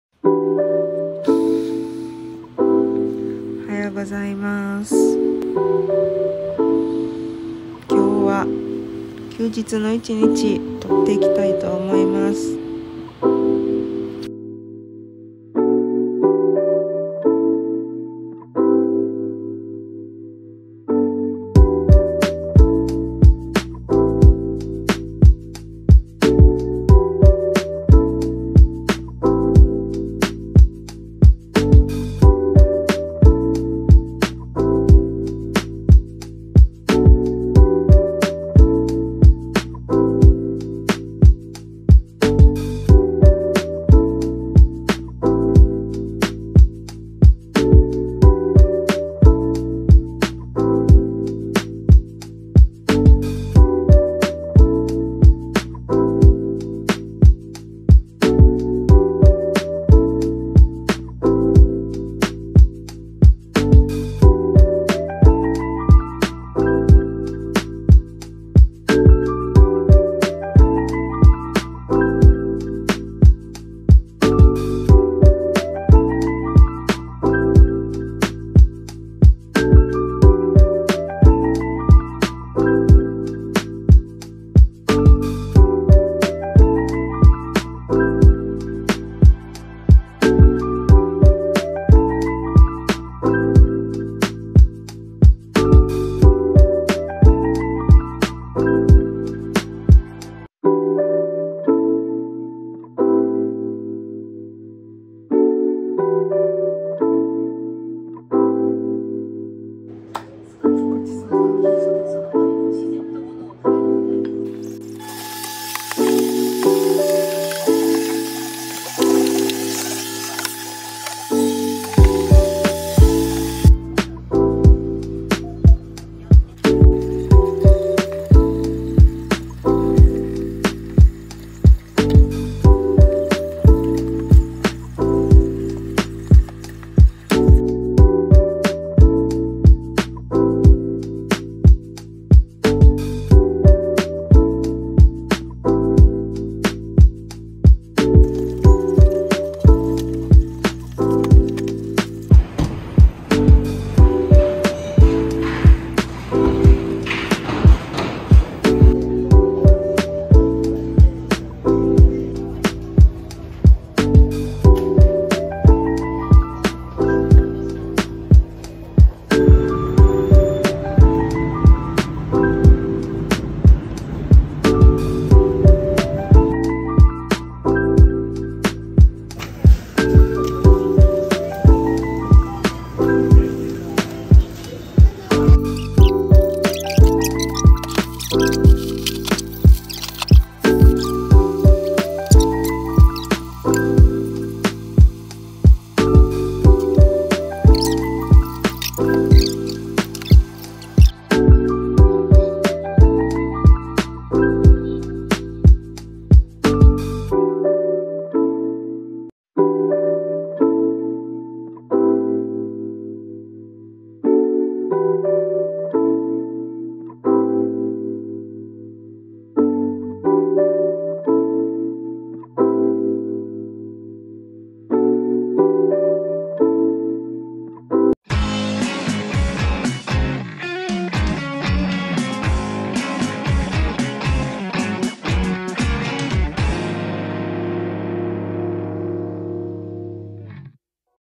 【密着】ヤングの休日
今日は東京に住んでいるヤングの休日に
1日密着します！！
最近コロナで自粛生活。仕事も在宅なので
平日はほんまに一歩も出ず休みの日に生活用品を買いに少し外に出て、ほんと辛い日々です。
早く地元に帰りたいしお出かけしたい
早く落ち着いてくれることを祈る毎日です。
皆さんも一緒に頑張って乗り越えましょうね🥺